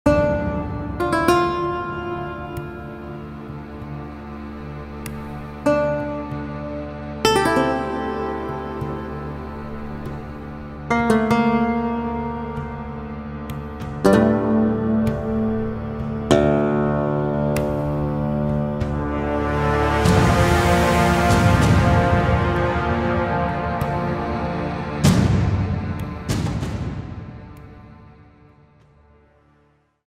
Motte and Bailey Castle - Animated Scene - Czepeku
Here is our next release for Czepeku Scenes!

This is the Motte and Bailey Castle!

We'll be releasing tons of variations and collaborating with different creators around the world to bring immersive, exciting splash art for your RPG games!